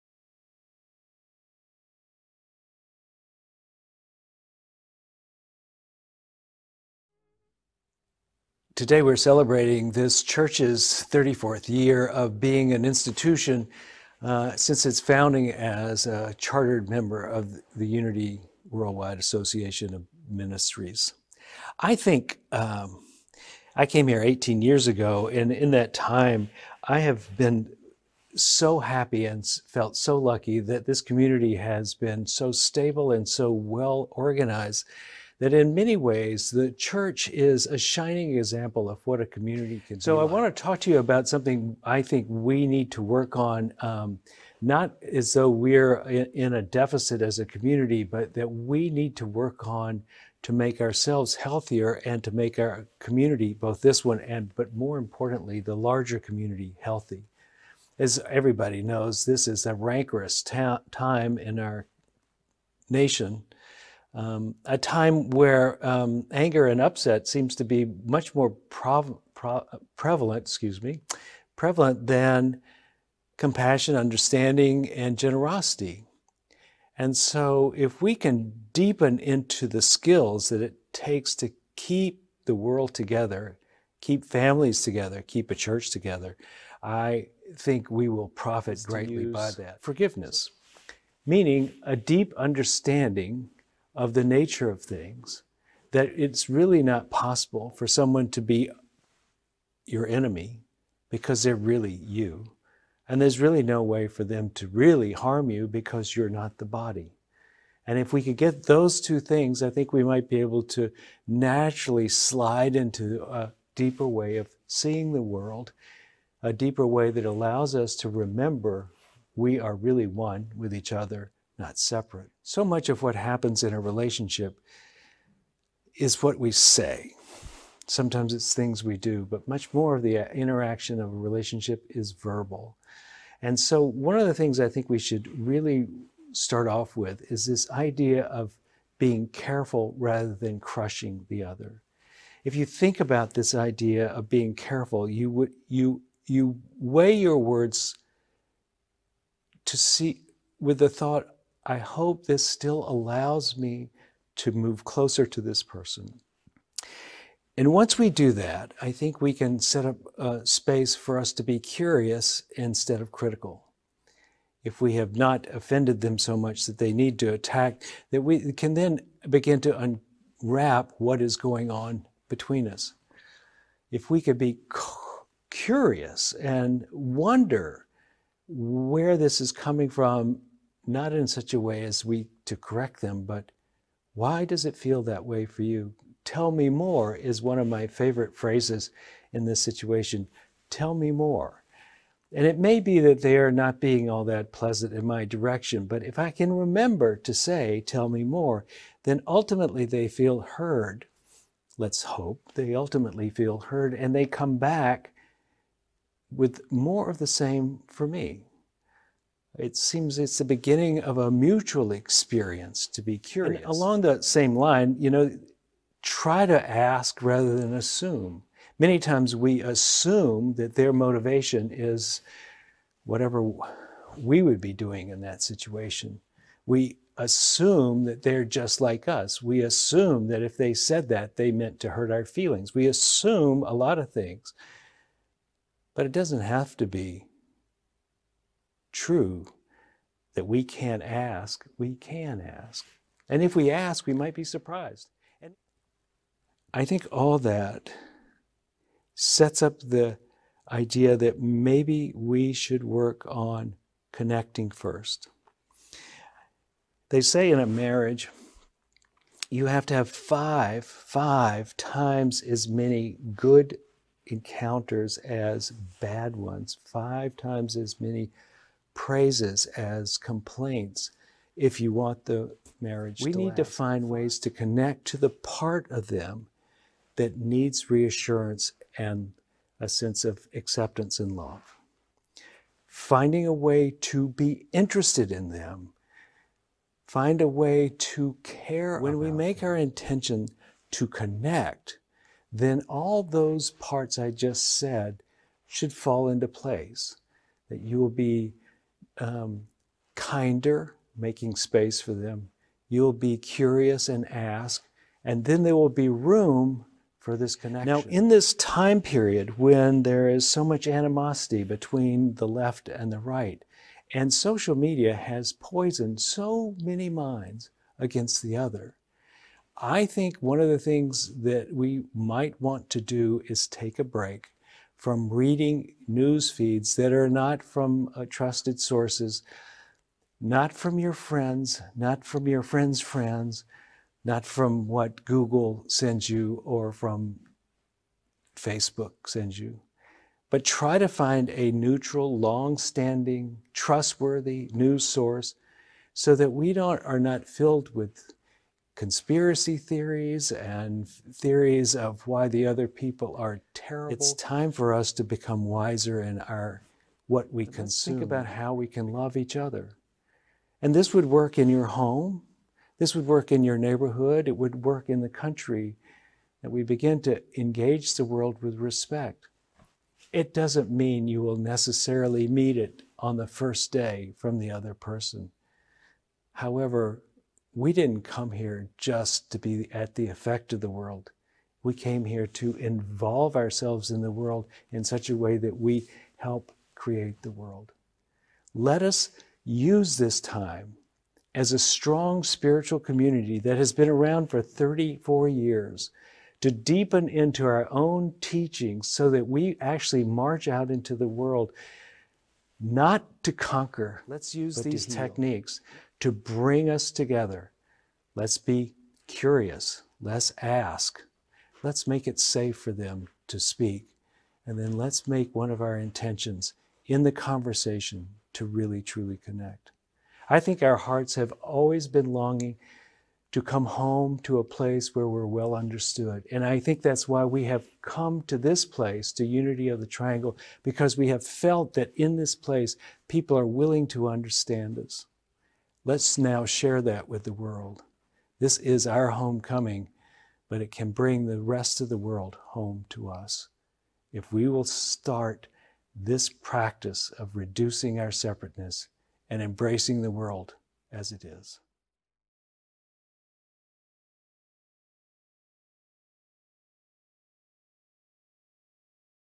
Sunday in 7 - Casting the Net of Love | Rev Neusom | Unity of the Triangle | Oct 18 2020
Casting a net of love to those around us will strengthen our relationships and emanate out into the world.  Forgiveness is key.  Being cautious with our words with others reduces feelings of defensiveness.  And being genuinely curious about their thoughts and feelings can start a productive diaglogue.  Making our intention one of connection with others will shine through and draw others towards us.  Let's cast our net of love out into the world to heal divisions and live out the truth of being one with each other.